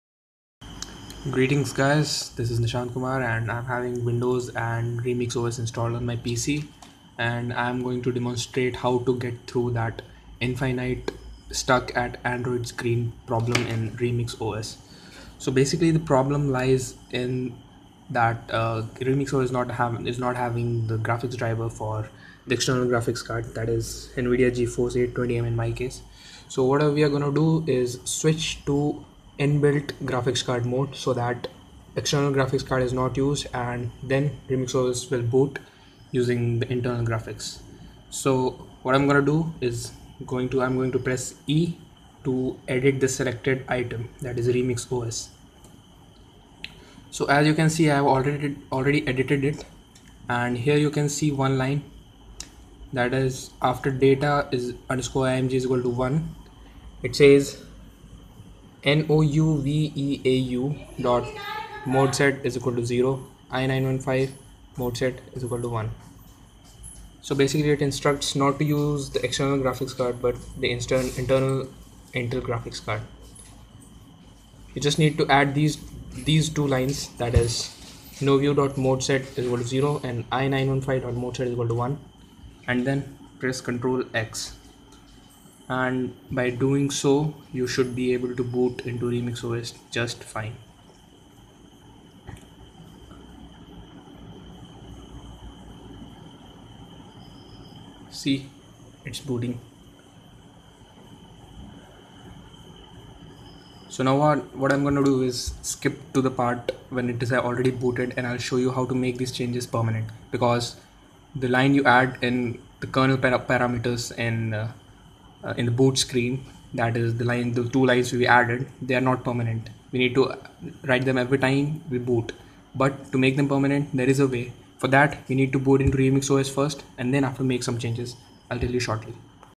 RemixOS Stuck At ANDROID Screen Solution Part1
RemixOS indefinitely stuck at A N D R O I D screen solution.
Paste the following in any text editor while booted in RemixOS. Save the file. Rename the file as grub.cfg and place it in the directory shown in the video.

set timeout=30
set gfxmode=1024x768
terminal_output gfxterm

menuentry 'Windows' {
 search --file --no-floppy --set=root /EFI/Microsoft/Boot/bootmgfw.efi
 chainloader (${root})/EFI/Microsoft/Boot/bootmgfw.efi

menuentry 'Remix OS' --class android-x86 {
    insmod /efi/RemixOS/ntfs64.mod
    insmod /efi/RemixOS/exfat64.mod
 search --file --no-floppy --set=root /RemixOS/kernel
 linuxefi /RemixOS/kernel root=/dev/ram0 androidboot.hardware=remix_x86_64 androidboot.selinux=permissive quiet DATA= SRC=RemixOS CREATE_DATA_IMG=1
nouveau.modeset=0 i915.modeset=1
 initrdefi /RemixOS/initrd.img